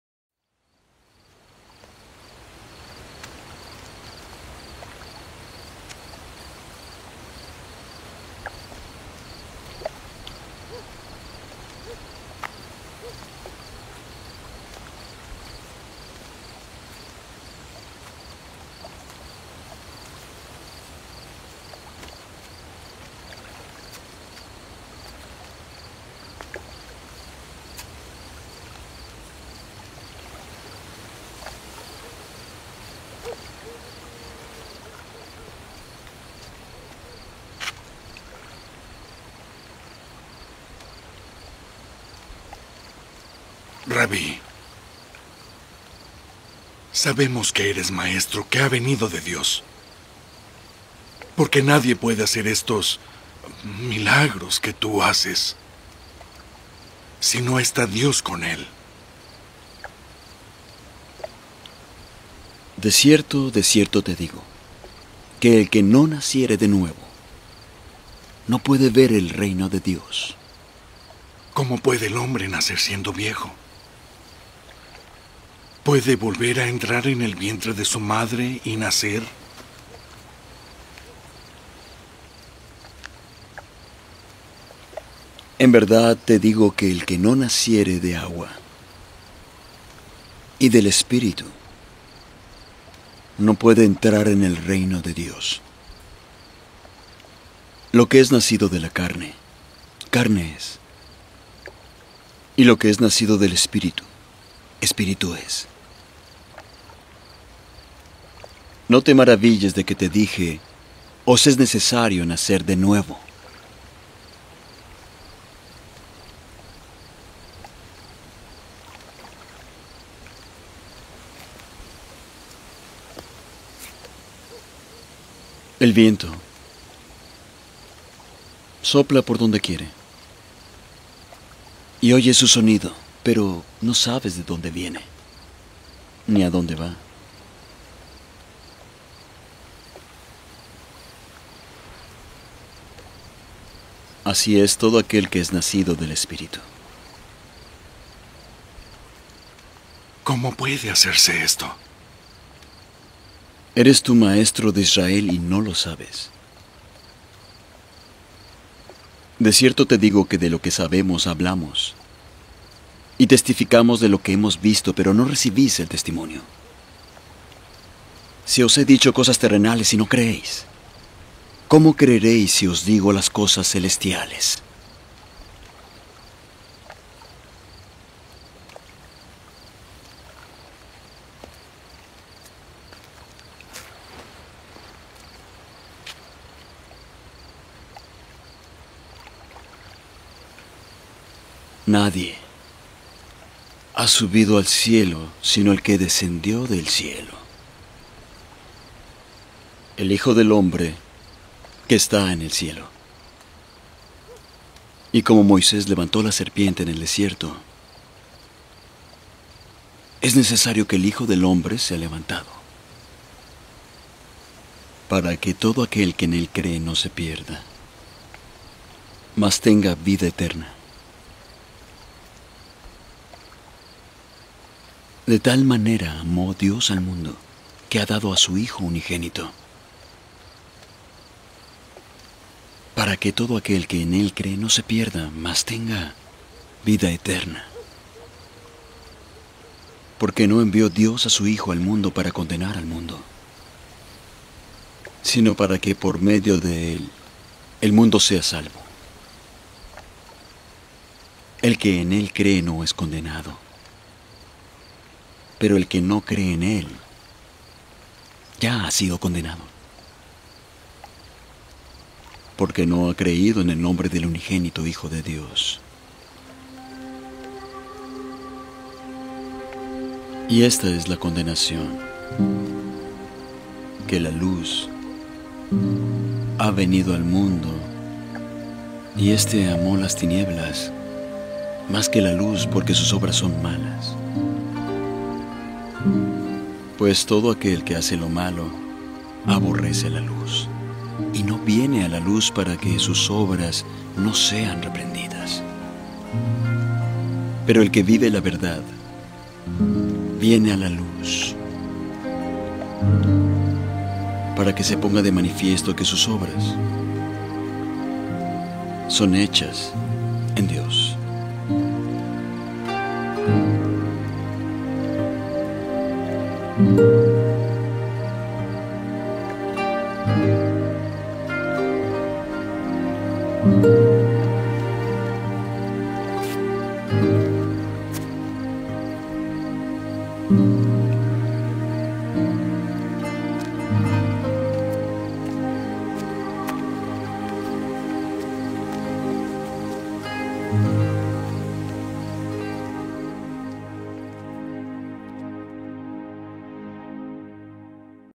JESÚS DE NAZARETH 2 Y SUS ENSEÑANZAS Acerca de Nacer de Nuevo😀👍 - Algo muy Tierno y Sincero😉👈
El que no Naciere de Nuevo. No puede Ver El Reino de DIOS.🤣😉
El que no Naciere de Agua y Espiritú. No podra Entrar en el Reino de DIOS.😂😉
Lo que es Nacido de Carne. Es Carne.😉😉
Lo que es Nacido de Espiritú. Es Espiritú.😀🤣
No te Maravilles de te Dije. Os es Necesario Nacer de Nuevo.😂😀

Esperó que les guste y no olviden qué los Quiero Mucho. ❤️💙💚💛😉👍

He who is not Born Again. Cannot See The Kingdom of GOD.🤣😉
He who is not born of Water and Spirit. He will not be able to enter the Kingdom of GOD.😂😉
What is Born of Flesh. Is Flesh.😉😉
What is Born of Spirit. Is Spirit.😀🤣
Do not marvel at what I told you. You must be born again.😂😀

I Hope you like it and don't forget that I love you very much. ❤️💙💚💛😉👍